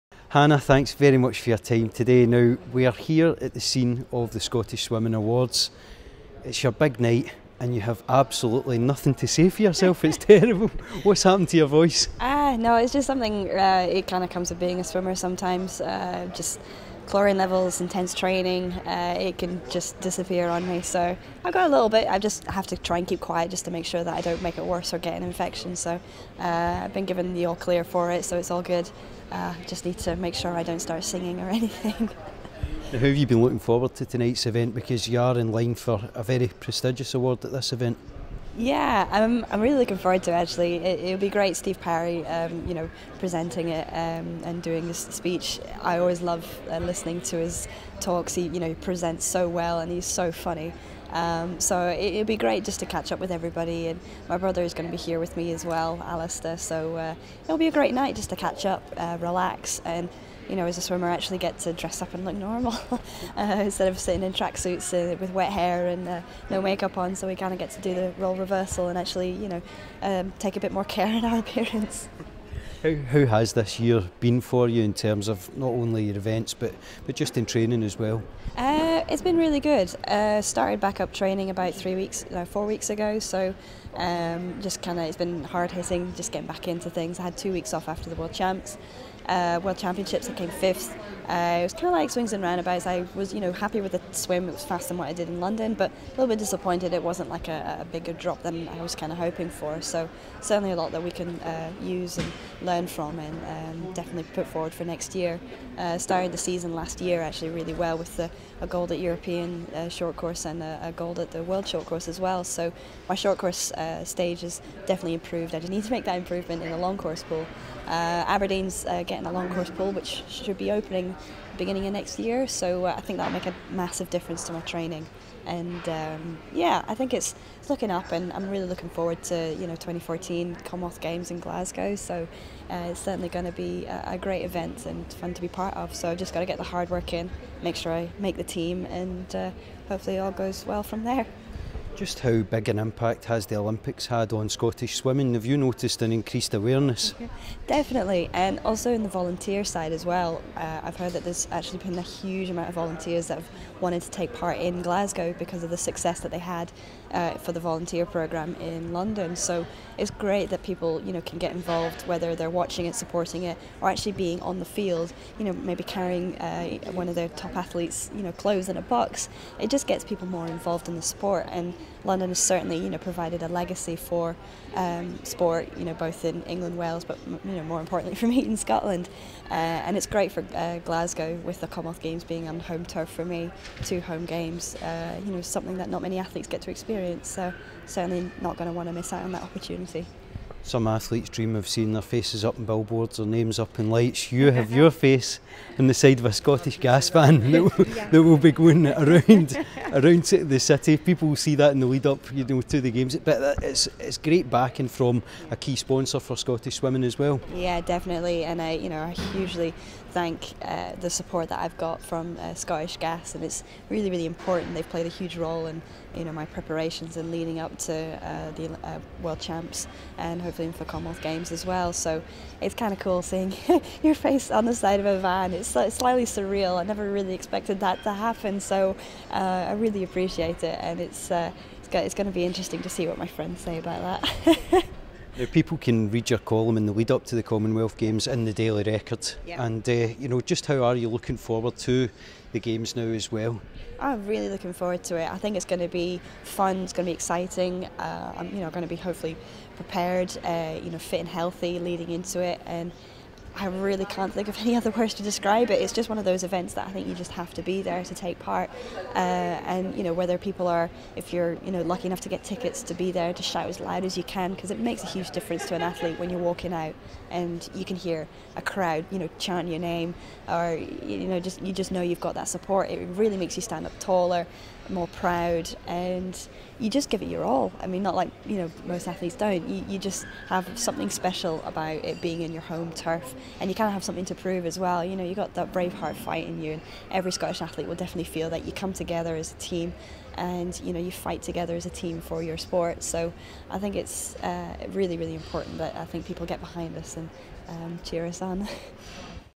Hannah Miley Scottish Swimming Awards 2013
Gregor Kyle of the Scottish Daily Record speaks to swimmer Hannah Miley at the Scottish Swimming Awards 2013 at the Glasgow Science Centre